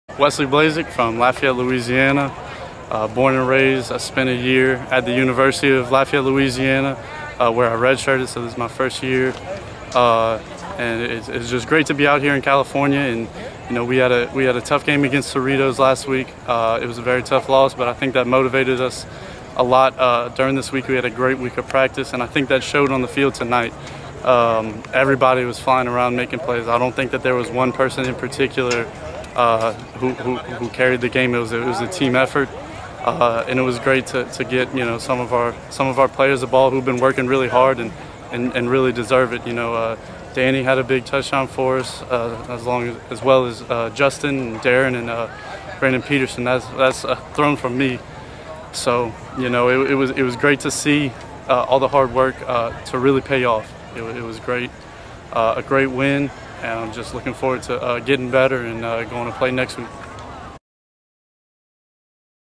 Wesley Blazek - Quarterback East L.A College Huskies Football vs Orange Coast College 2019
Quarterback Wesley Blazek found his groove in the first half, passing for four touchdowns to four different receivers (finishing 20 for 33, 252 yards and four touchdowns). Daniel Munguia led all receivers with 9 catches for 122 yards and a touchdown, with Justin Watkins, Branden Peterson, Darren Jones and Emajea Porter also catching touchdown passes (Jones with two TD catches).